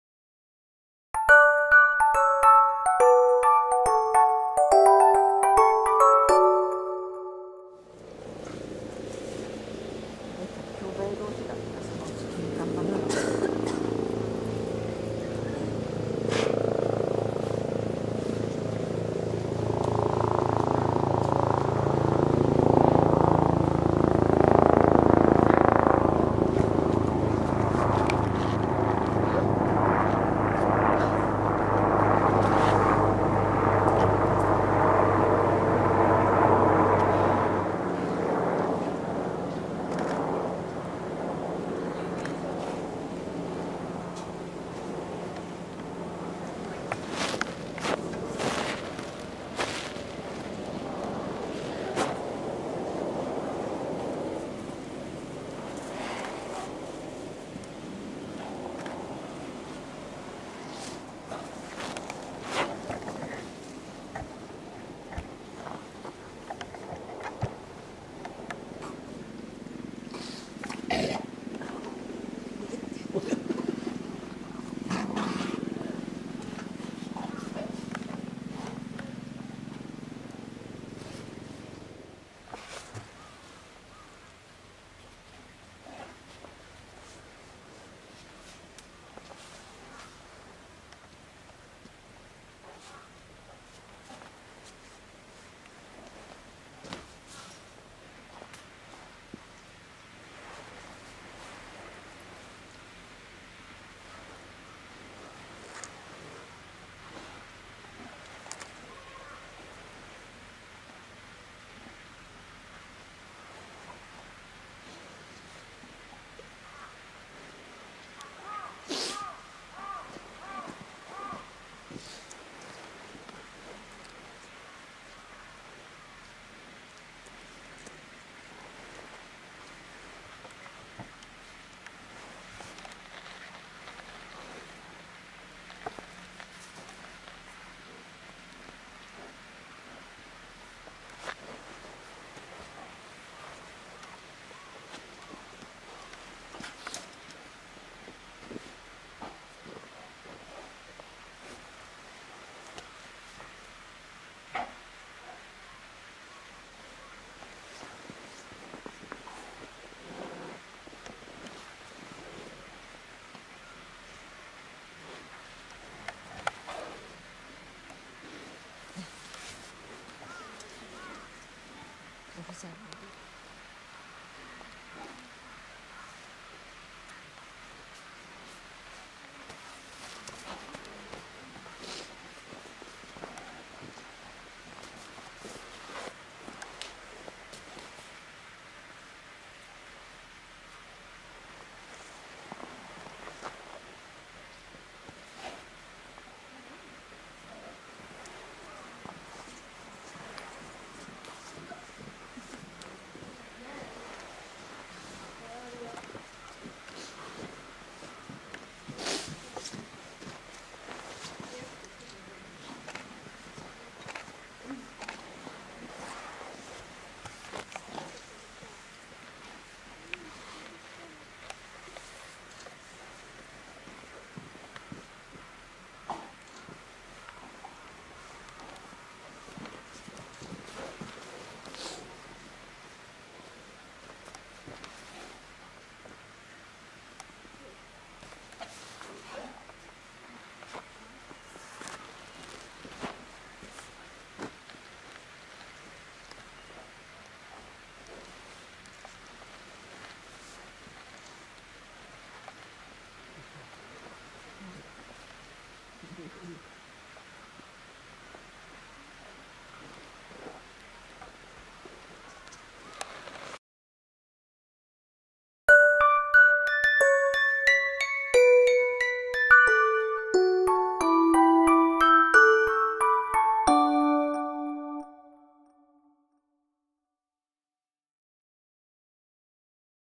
Polar Bear Wrestling 首絞め過ぎちゃったララ
Polar Bears,Lara (mother/21) & Lila  (daughter/1).
円山動物園のホッキョクグマ、ララ(メス/21歳)、リラ(メス/1歳)。
日課の親子レスリング。
リラの力もついてきているので、ララも少し大変そうです。
(2016年1月8日(金)　北海道札幌市　円山動物園にて撮影)